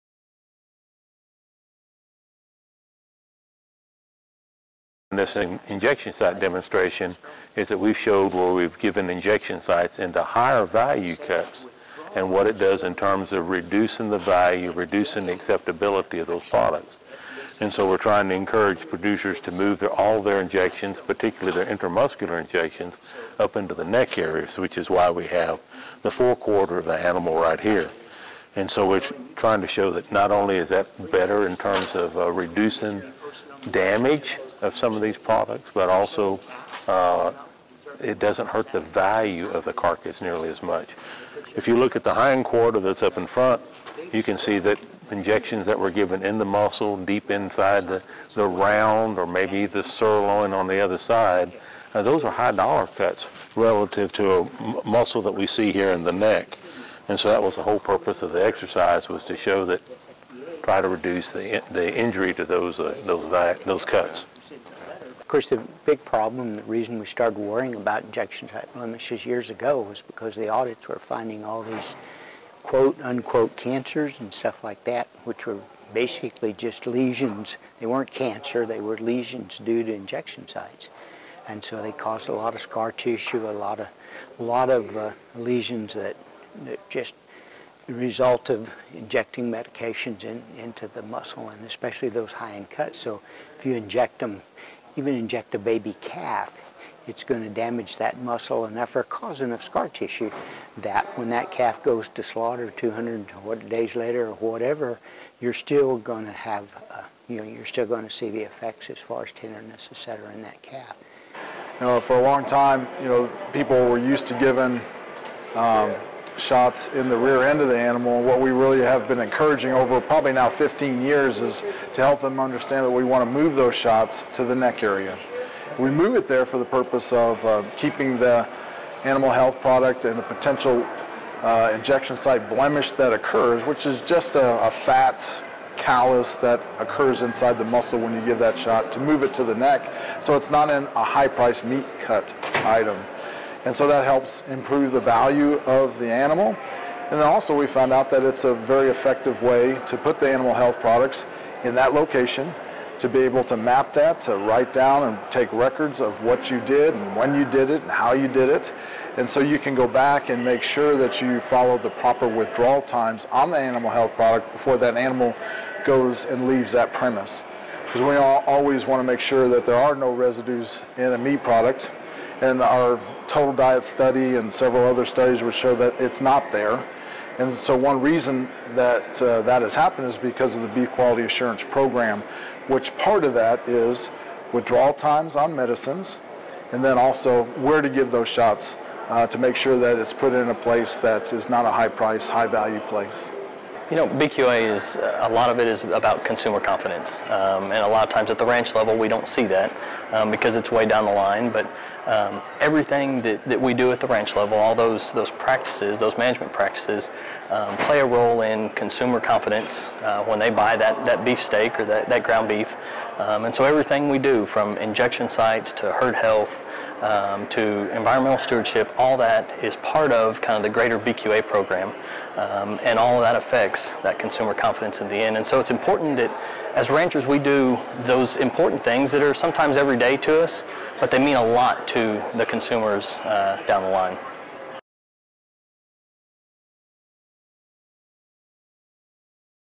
Value of Proper Injection Sites
A team of Texas A&M AgriLife Extension Service specialists demonstrated how much injection site blemishes can diminish the value of high-end cuts on the beef carcass at the recent Texas and Southwestern Cattle Raisers Association Convention in Fort Worth.
Dr. Joe Paschal, AgriLife Extension livestock specialist from Corpus Christi, was joined by Dr. Dan Hale, AgriLife Extension meat specialist, and Dr. Tom Hairgrove, Extension specialist, all from College Station, who gave a demonstration on the value of proper injection sites on cattle.